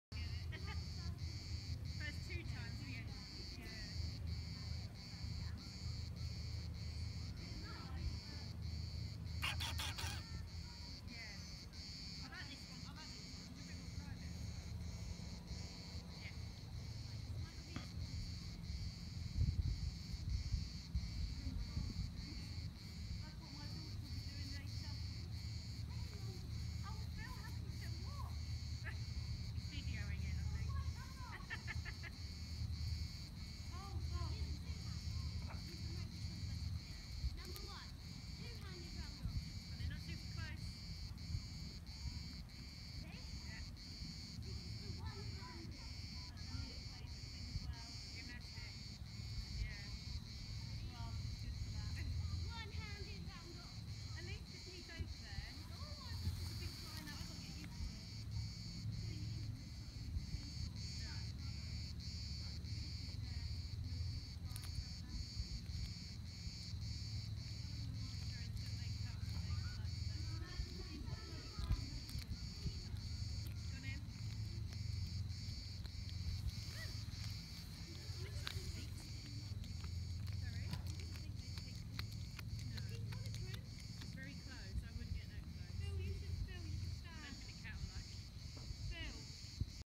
Cat versus Snake
Cat finds a new toy, and the crunching at 1:06 is gruesome!